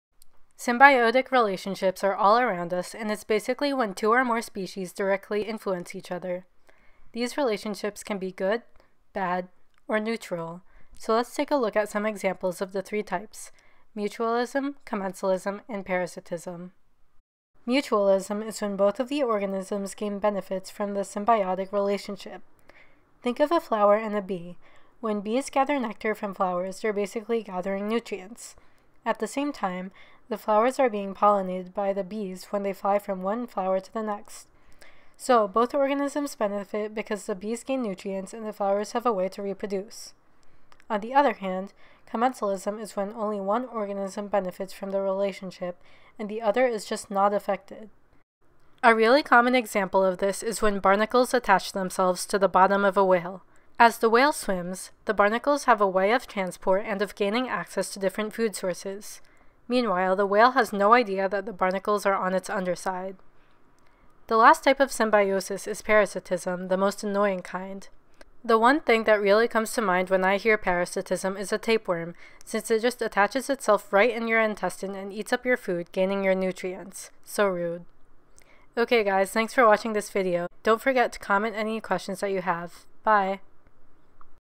Symbiosis
This video explains symbiotic relationships such as mutualism, commensalism, and parasitism.

*** If there are any pictures used in this video, they are NOT MINE and I will not take credit for them. ***

TRANSCRIPT:
Symbiotic relationships are all around us, and it’s basically when two or more species directly influence each other. These relationships can be good, bad, or neutral, so let’s take a look at some examples of the three types: mutualism, commensalism, and parasitism.
Mutualism is when both of the organisms gain benefits from the symbiotic relationship. Think of a flower and a bee. When bees gather nectar from flowers, they’re basically gathering nutrients. At the same time, the flowers are being pollinated by the bees when they fly from one flower to the next. So, both organisms benefit because the bees gain nutrients and the flowers have a way to reproduce.
On the other hand, commensalism is when only one organism benefits from the relationship, and the other is just not affected. A really common example of this is when barnacles attach themselves to the bottom of a whale. As the whale swims, the barnacles have a way of transport and of gaining access to different food sources. Meanwhile, the whale has no idea that the barnacles are on its underside.
The last type of symbiosis is parasitism, the most annoying kind. The one thing that really comes to mind when I hear parasitism is a tapeworm, since it just attaches itself right in your intestine and eats up your food, gaining your nutrients.
Don’t forget to comment any questions that you have! Bye!